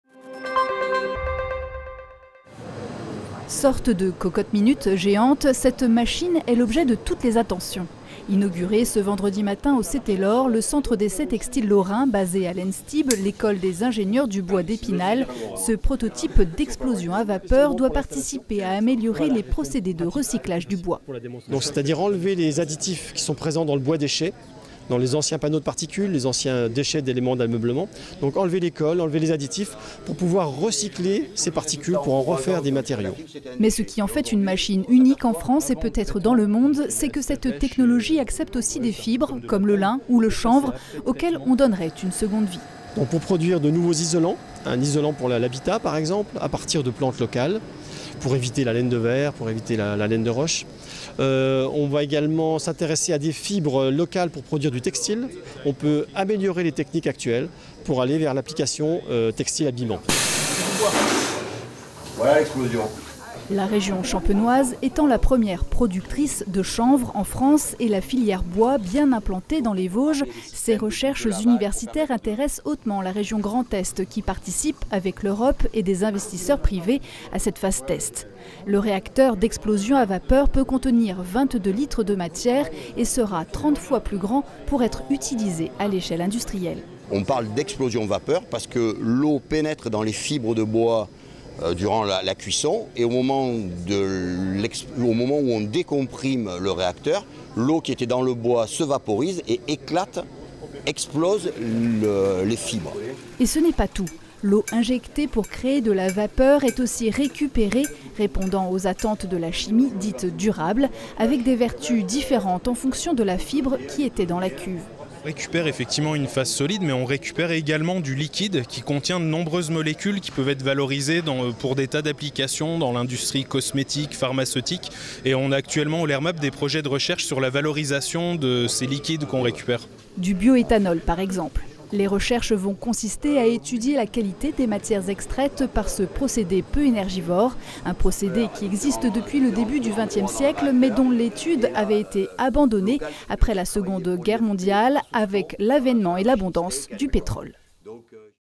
Explosion à vapeur
Inauguration ce vendredi matin au Cetelor, le Centre d'Essais Textile Lorrain situé au sein de Enstib, l'école des ingénieurs du bois d'Epinal, d'un prototype de machine d'explosion à la vapeur qui doit faciliter le recyclage du bois et des fibres textiles.